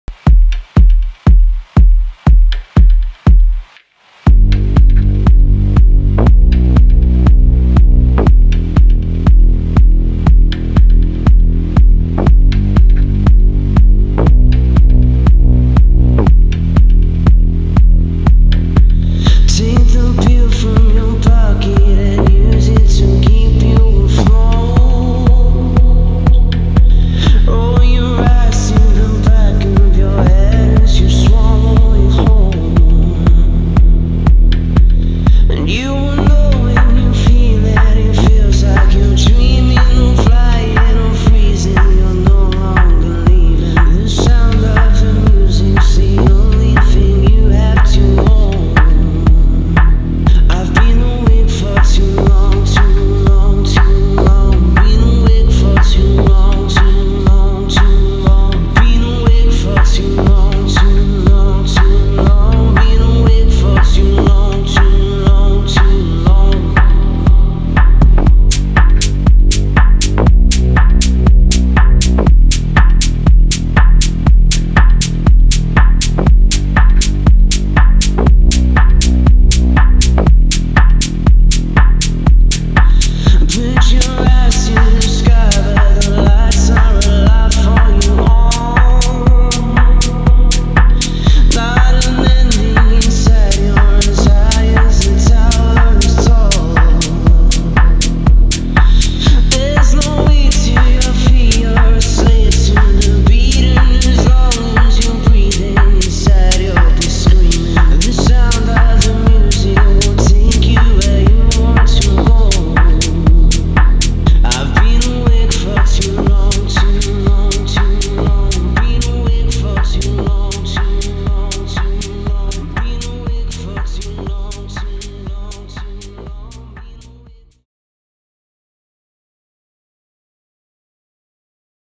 Santé @Logika Start The Party 20/12/2014
Santé [Avotré] ospite alla serata Logika at Shabba Club - Cantù (CO)
Berlinese di fama internazionale, produttore per diverse etichette tra le più famose al mondo, nel suo albumtour courrent, è stato protagonista di una grande serata.
Next Date 17/01/2015 Guest dj WEHBBA [From Brazil]